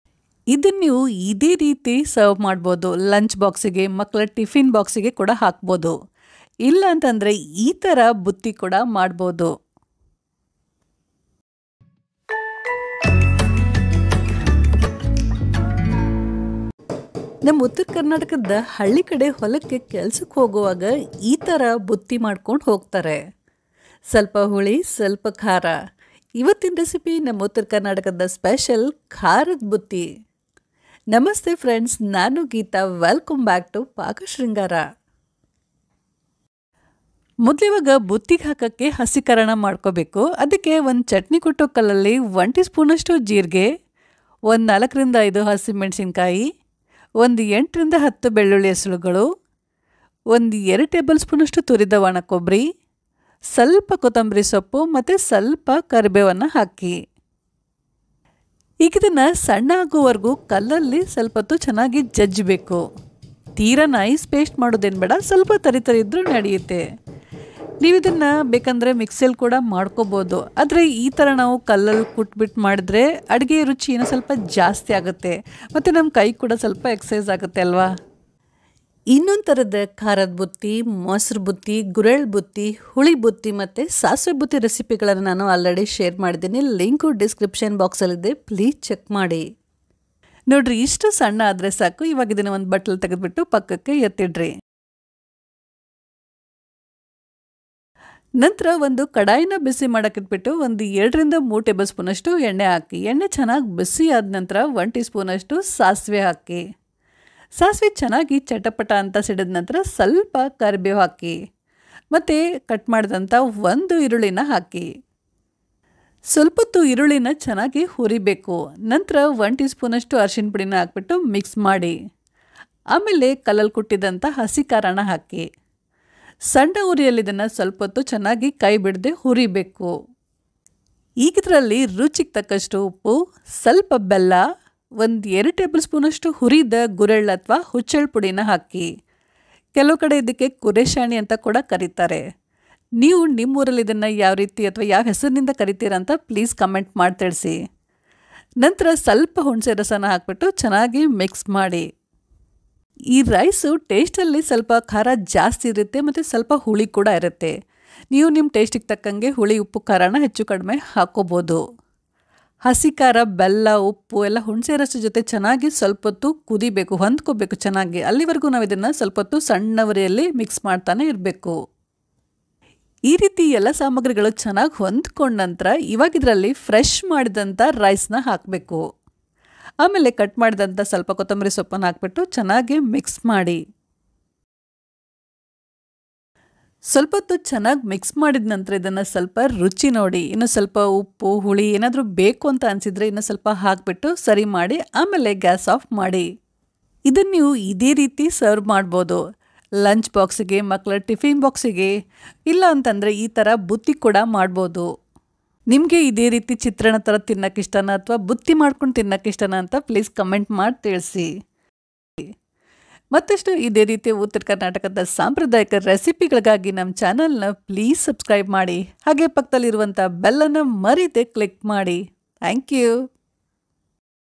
ಹಳ್ಳಿ ಶೈಲಿಯ ಖಾರದ ಬುತ್ತಿ| ಉತ್ತರ ಕರ್ನಾಟಕದ ಸ್ಪೆಶಲ್|Butti recipe|Spicy rice balls|Village style rice
Kharada butti recipe/Butti recipe in kannada/how to make Village style spicy Butti/Rice recipe/lunchbox recipe/UttaraKarnataka recipes/ಬುತ್ತಿ ಮಾಡುವ ವಿಧಾನ/ಖಾರದ ಬುತ್ತಿ

Paakashringar brings to you North Karnataka Special traditional rice recipe Butti (Spicy rice balls) it's very easy to prepare and very tasty also. You can store this rice for 1 day ,it's travel friendly recipe as well as a best recipe for lunchbox :)

Here's the list of Ingredients and theri measurements 👇
1 big below white rice
1 finely chopped onion
3-4 green chillies
1 tsp Cumin seeds
2 Tbsp grated dry coconut
8-10 Garlic cloves
2 Tbsp coriander leaves
8-10 curry leaves
salt to taste
2 Tbsp Roasted Niger seeds powder
1 tsp Turmeric powder
2-3 Tbsp oil
1 tsp Mustard seeds
jaggery
Tamarind paste as required

Please try this rice recipe and share your feedback in the comment section below.